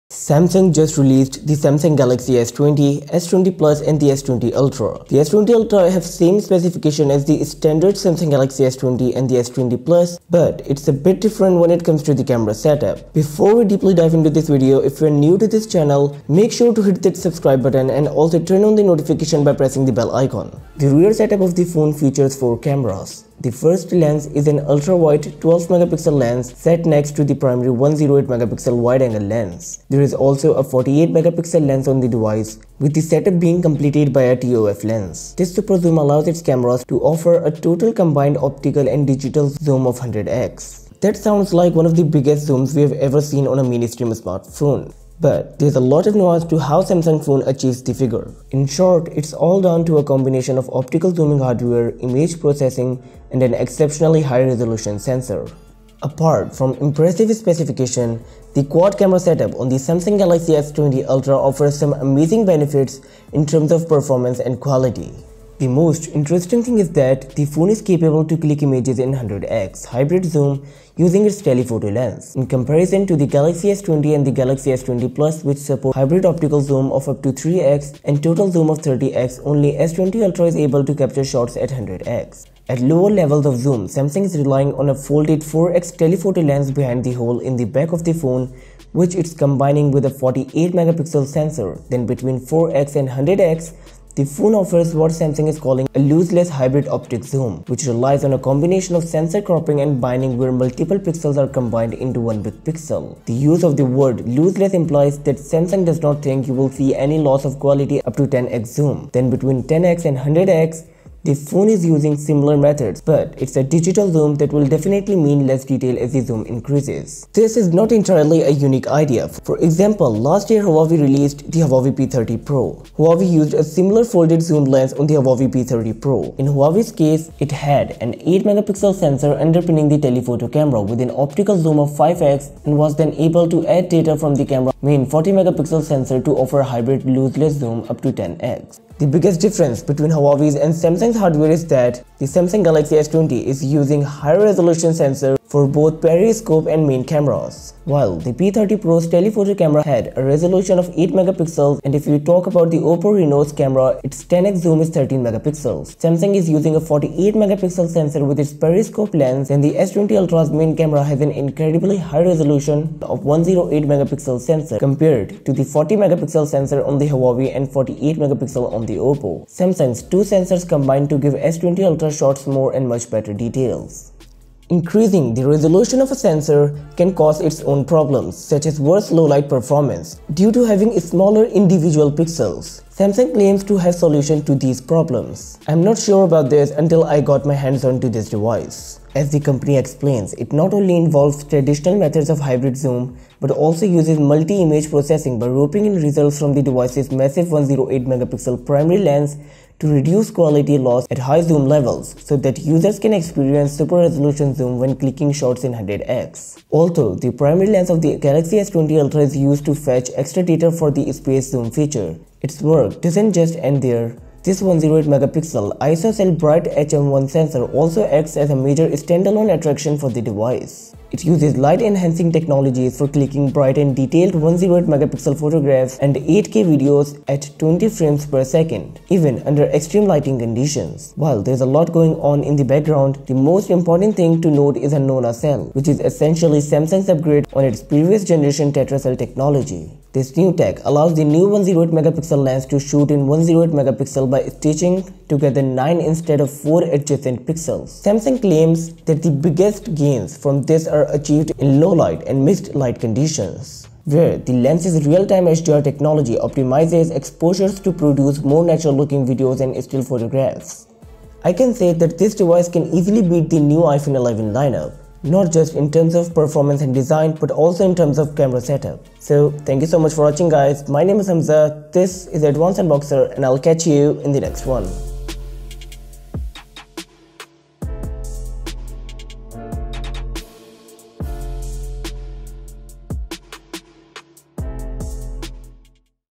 S20 Ultra 100X Space Zoom Explained | Advance Unboxer
In this video i explained how the Samsung Galaxy S20 Ultra's Space Zoom 100X is working. This might be one of the best smartphone of 2020.